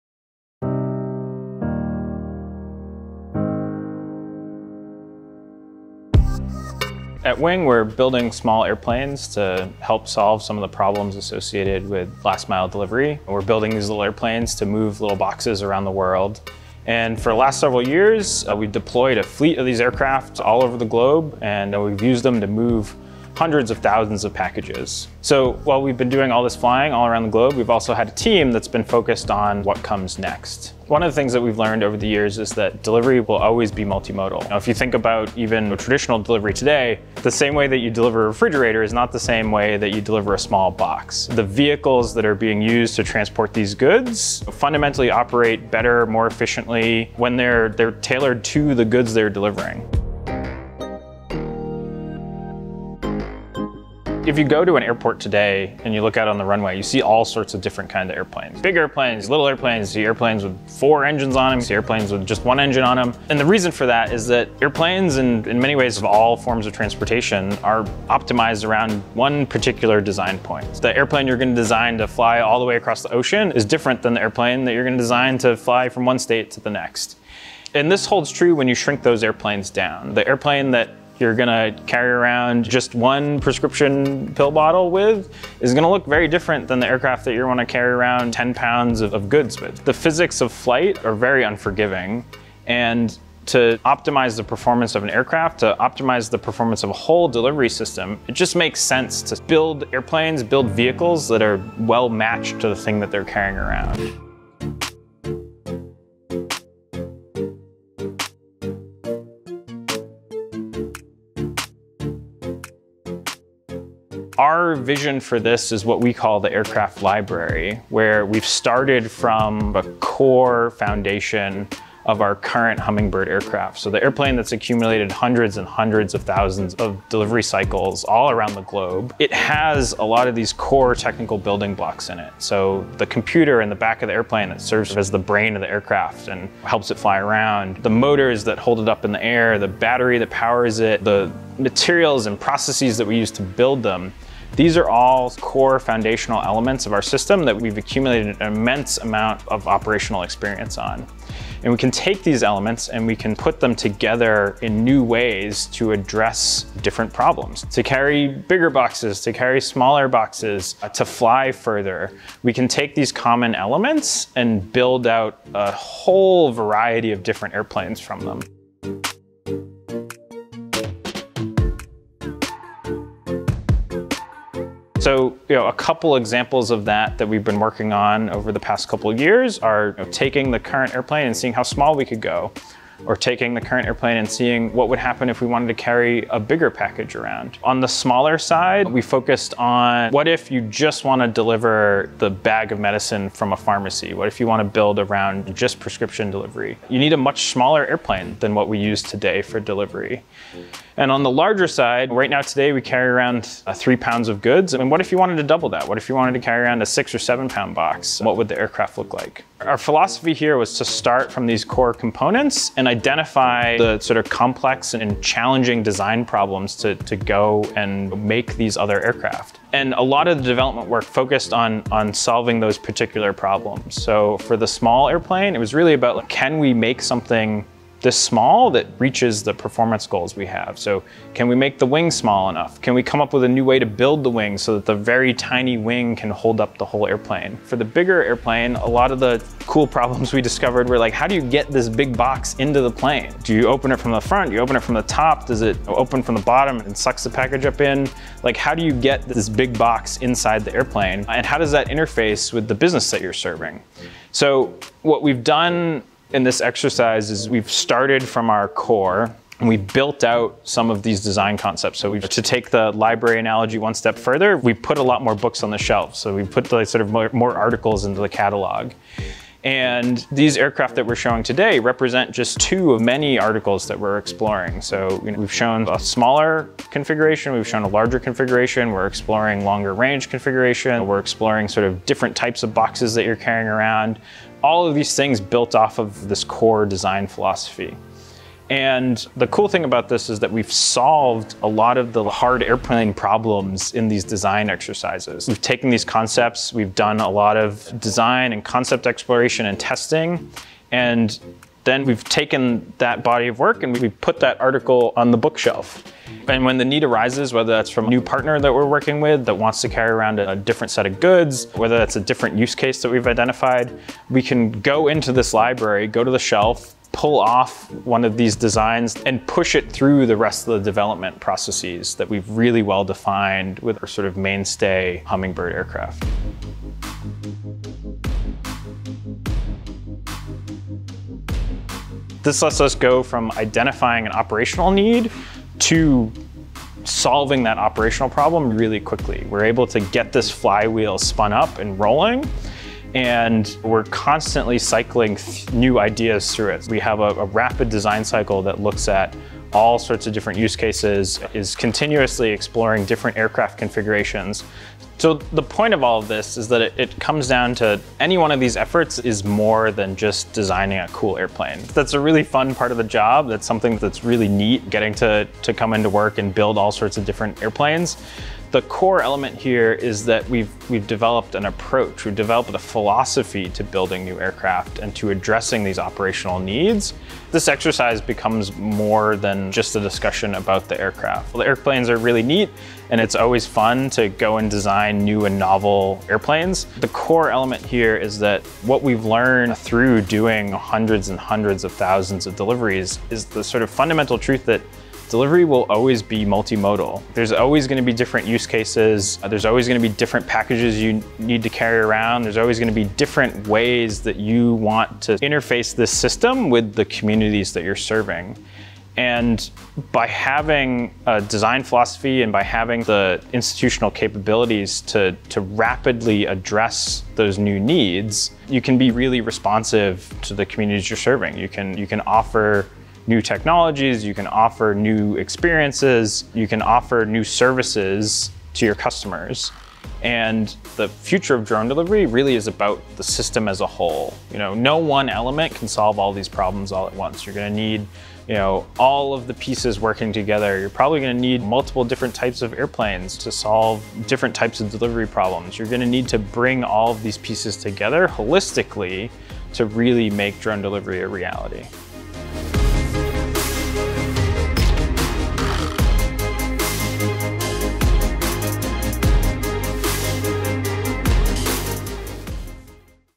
The Aircraft Library | Wing
Transportation has and will continue to be multimodal, and Wing’s design approach aims to address this. We’ve developed a core set of hardware and software components that can be used to create a variety of different vehicles tailored for specific use cases: an Aircraft Library. While we’ve been accumulating hundreds of thousands of delivery cycles on these core components in our Hummingbird series aircraft, we’ve also been filling the shelves of this library with additional configurations.

Read the official Wing Blog► blog.wing.com